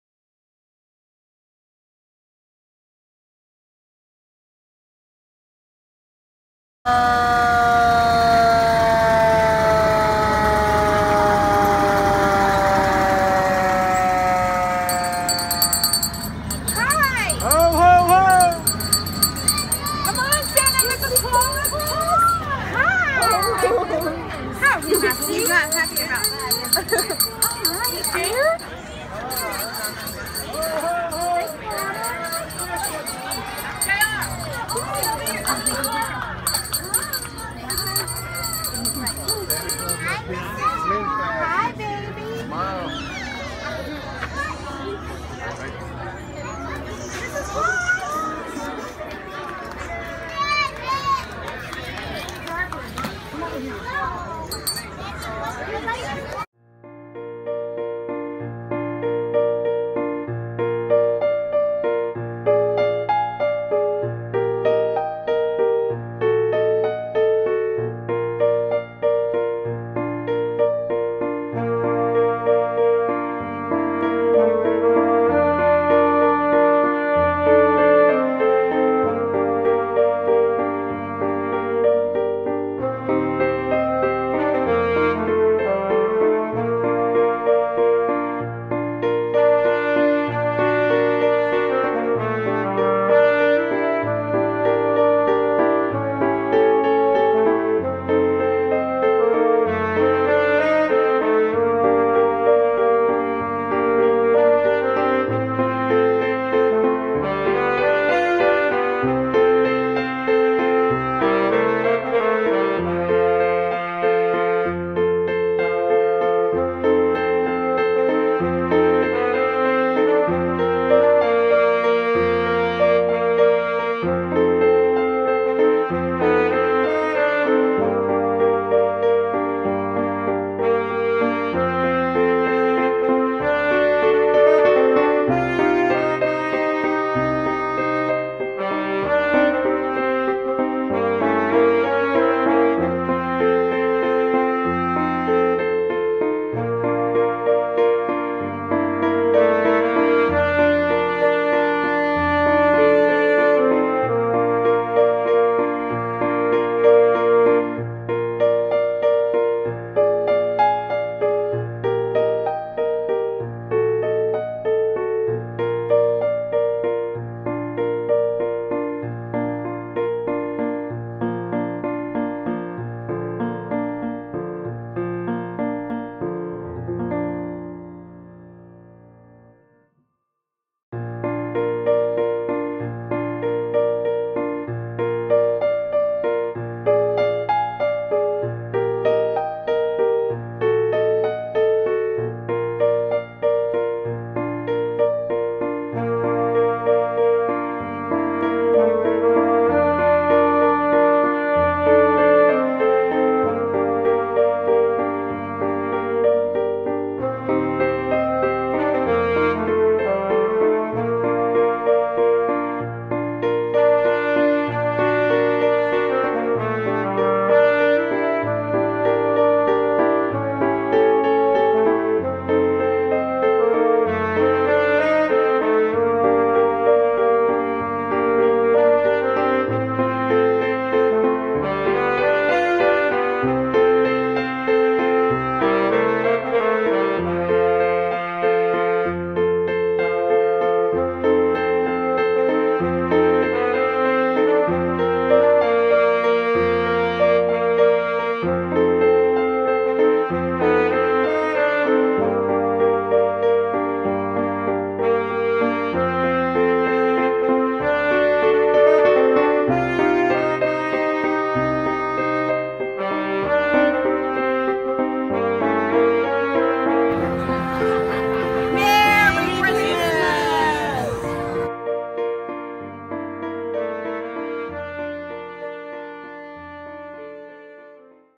Christmas Under the Stars 2015 and Freedom Veterans Exhibit featuring the Works of Lori Gomez
December 11 and 12, in Griffith Park in Olde Towne Slidell.